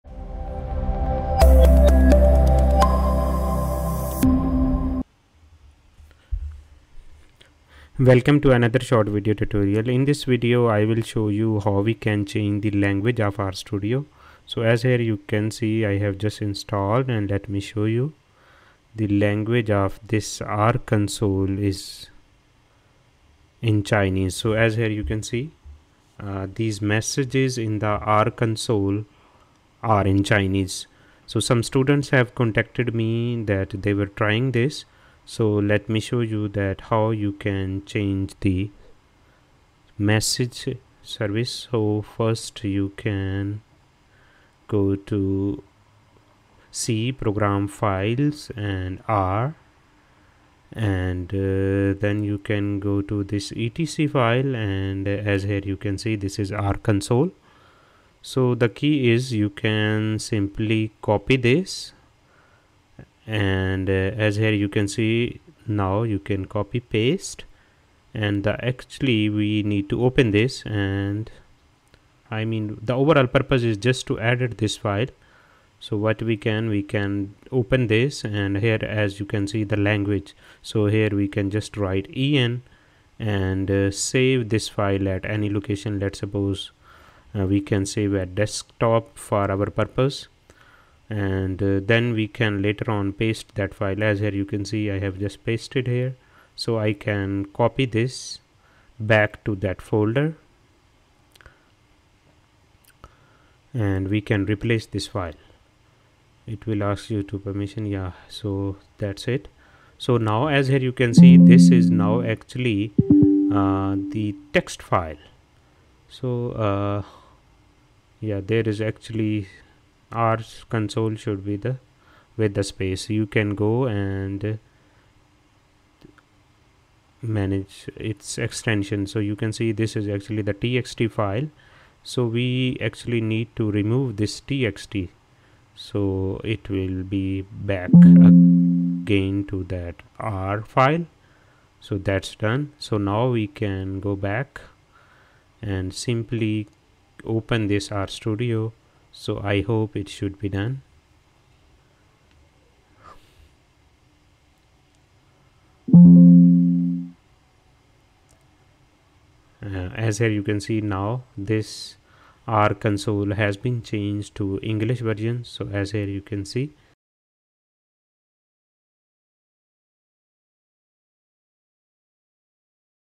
How to change language of messages in R console
In this video, I have shown how to change the language of R console messages from Chinese to english.
🔴If you are a graduate student and trying to learn something new easily, Watch my playlists